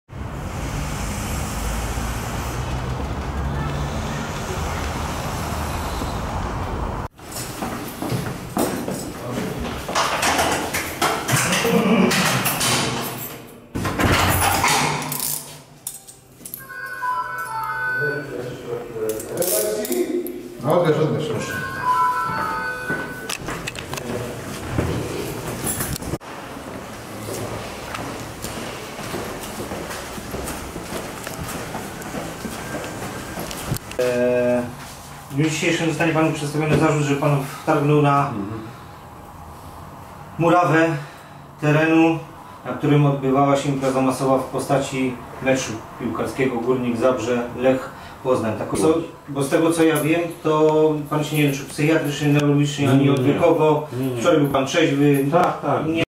Nagi kibic z meczu Górnika na komendzie - 28 lipca 2014
źródło: KMP Zabrze